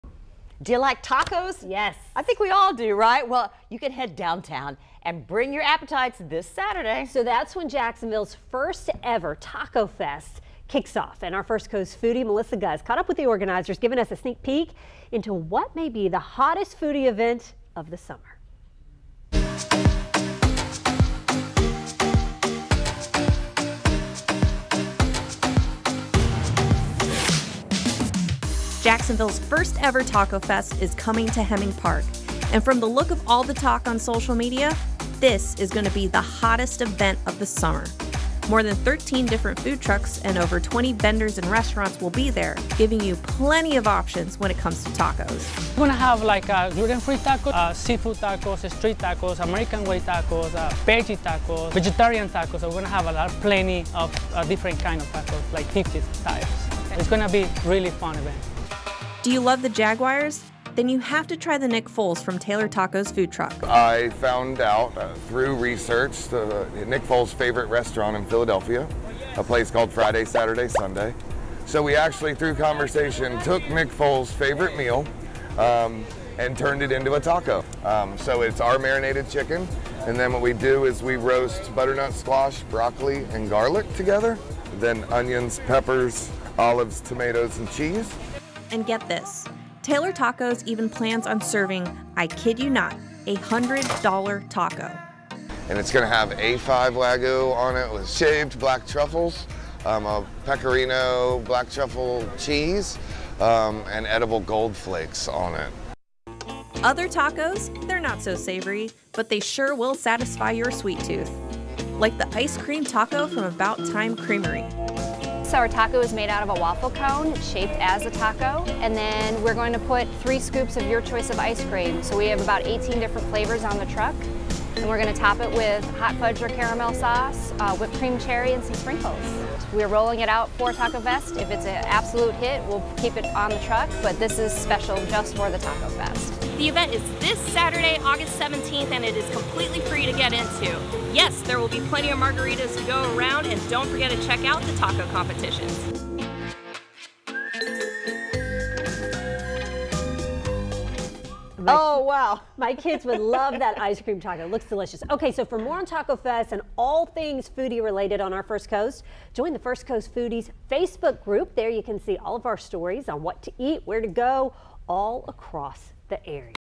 What you need to know about the first-ever Jacksonville Taco Fest
The Jax Taco Fest is on Aug. 17 from 1 p.m. to 7 p.m. Here's everything you need to know when it comes to what kinds of tacos will be available to parking.